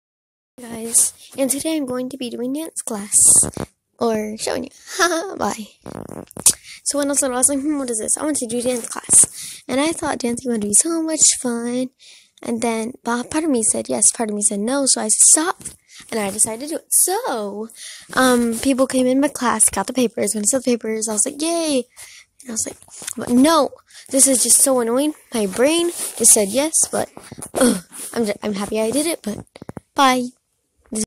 Dance Class Sign Up! Pt1
Hey guys! Turns out I signed up to class class in 5th grade.